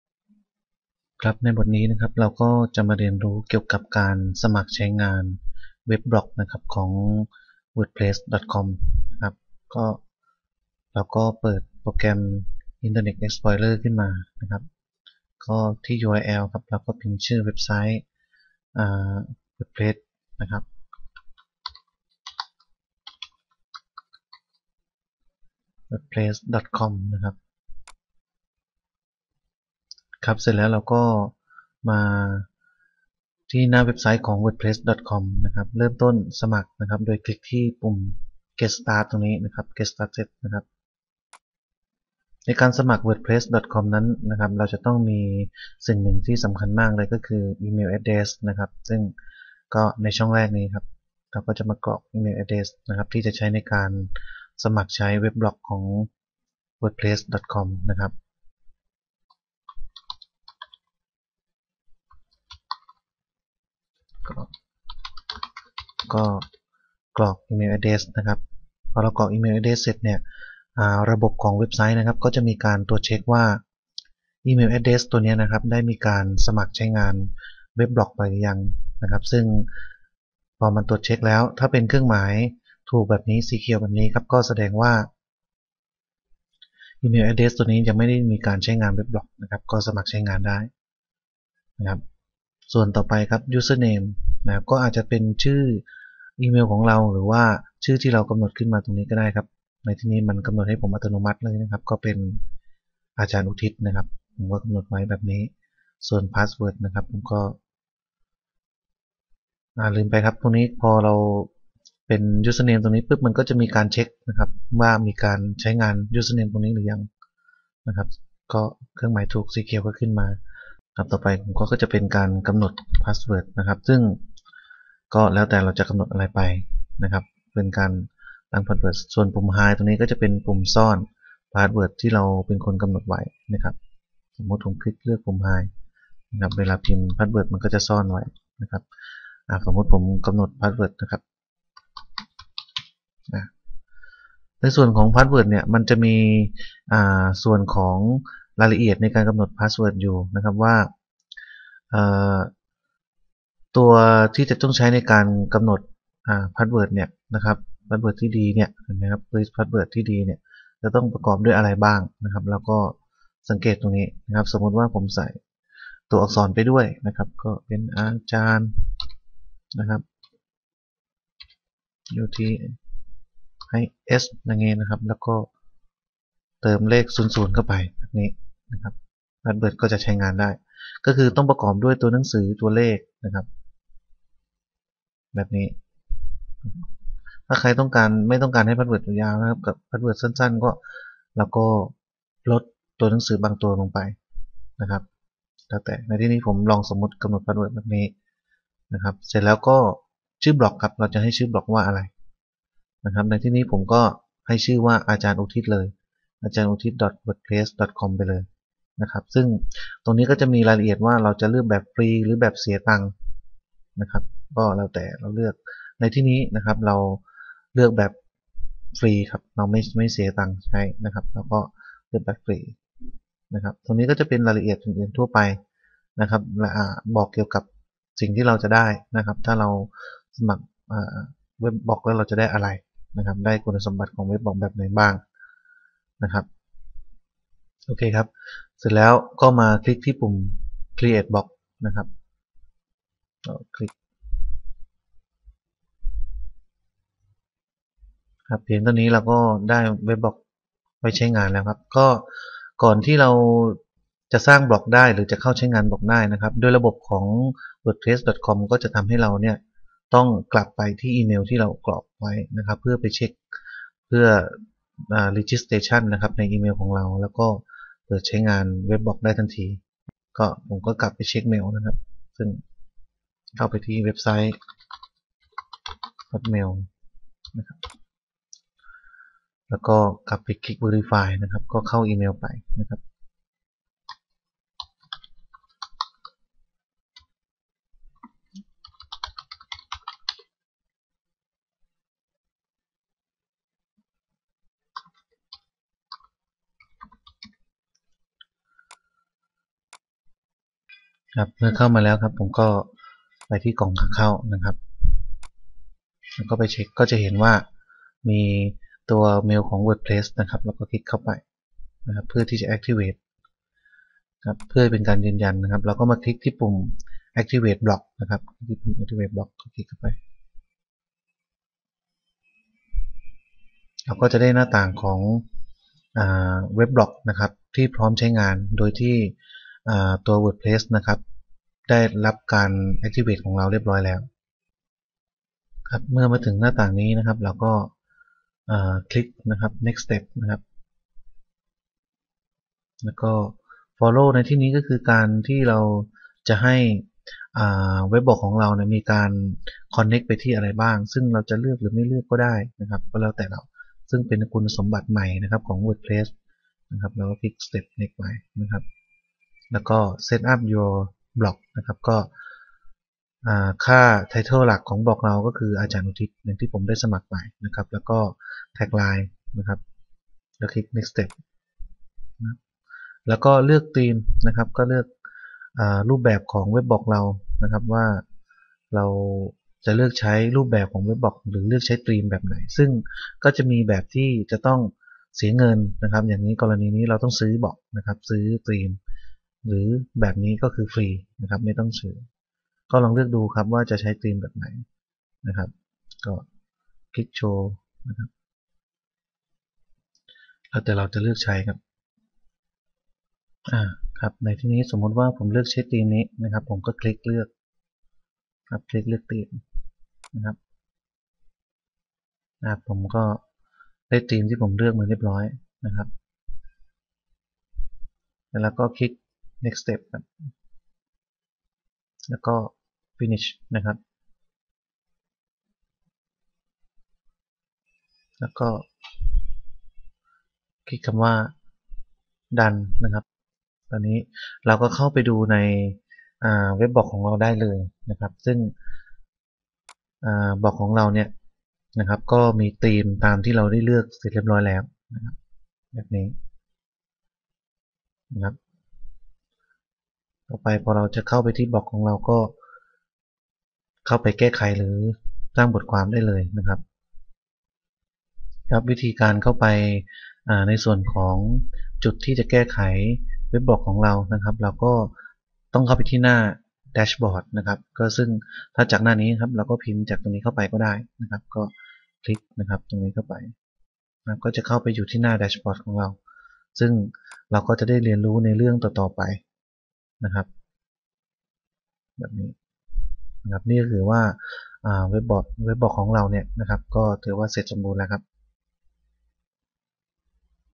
วิธีการสมัครใช้งาน Wordpress.com
แนะนำวิธีการการสมัครเข้าใช้งาน Wordpress.com
ดูรายการอื่น ๆ ได้ที่นี่